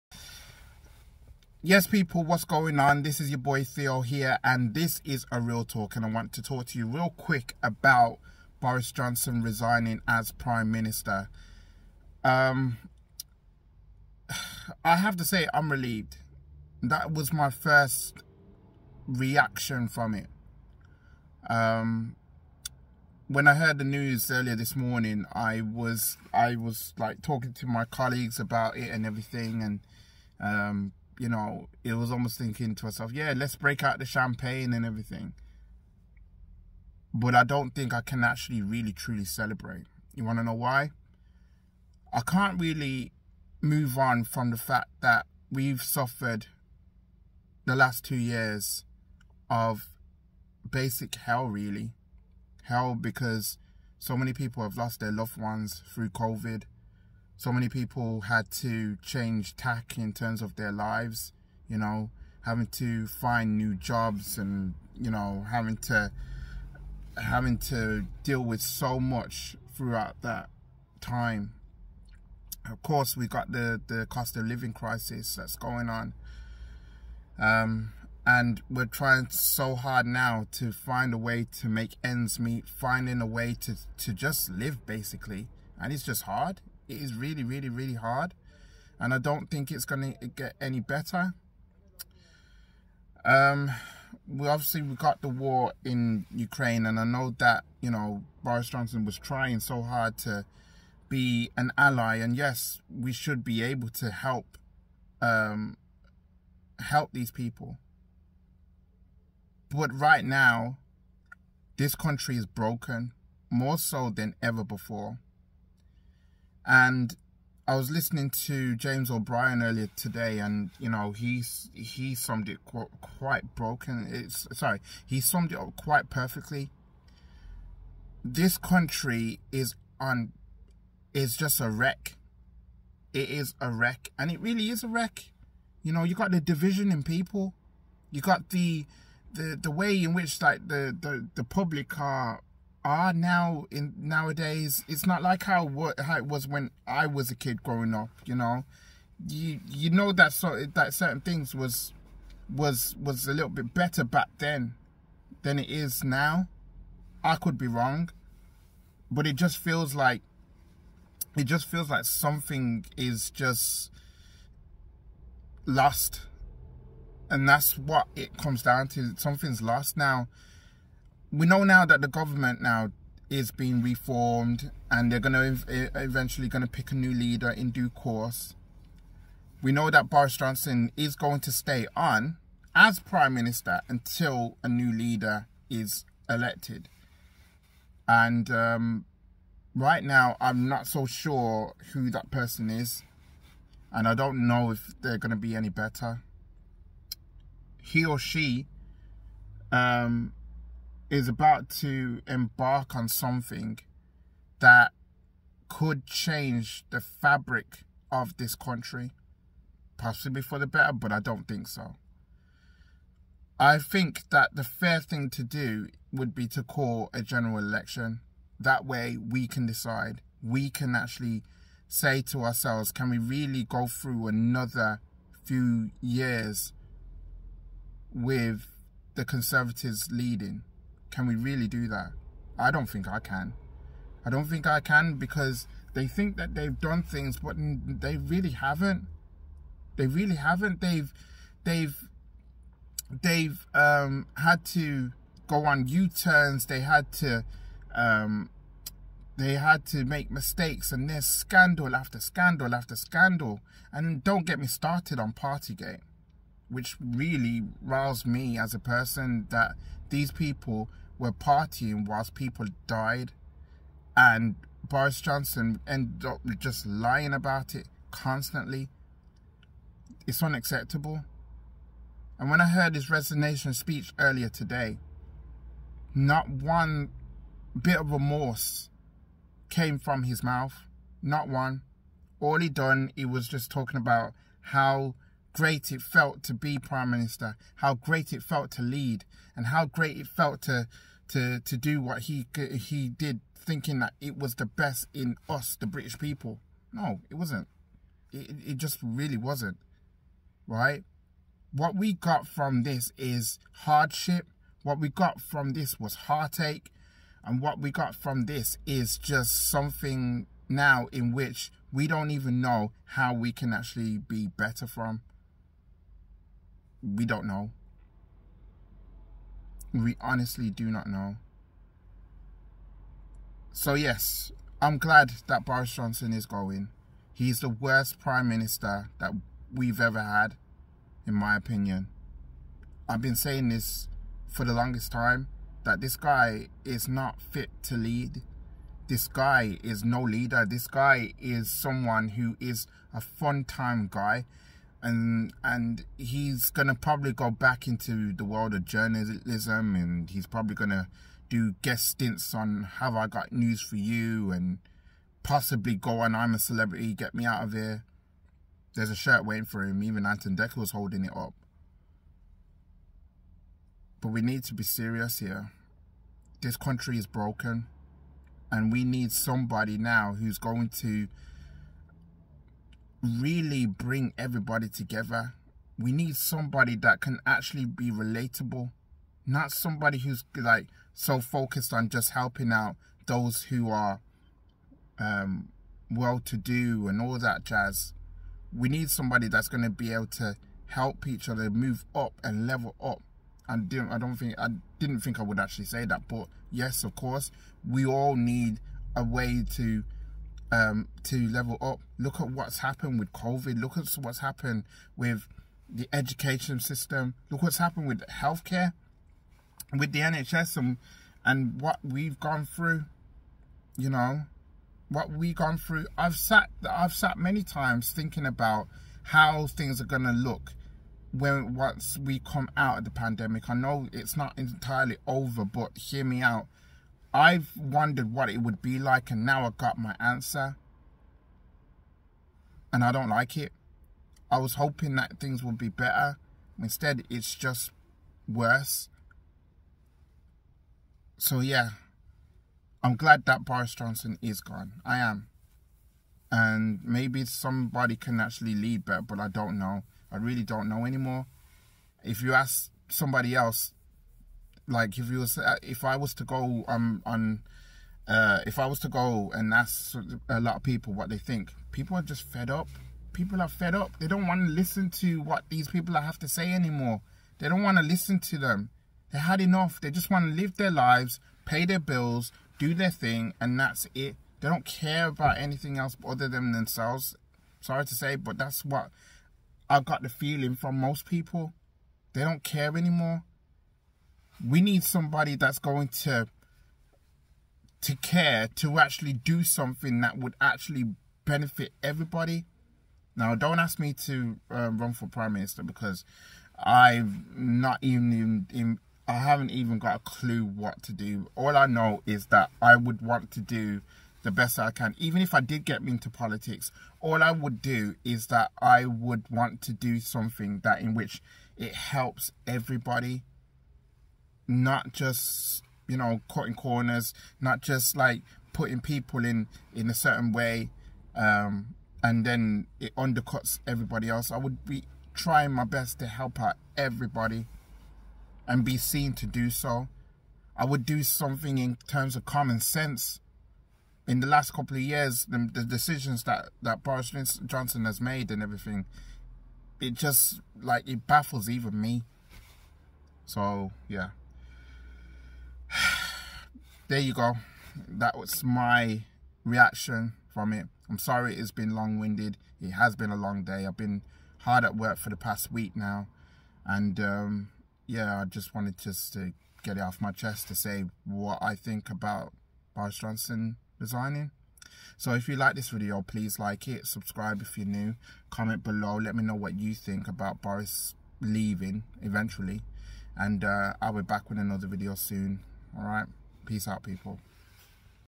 RealTalk - Boris Johnson Resigns As Prime Minister!
I have made no secret in stating that Boris should go and now it has happened. I should be dancing and singing in joy but I'm not. I'm relieved. I'm just exhausted to be honest. Scandal after scandal. He couldn't make the right decisions. He is no leader. I'm glad he's going to go. We need change. We deserve better.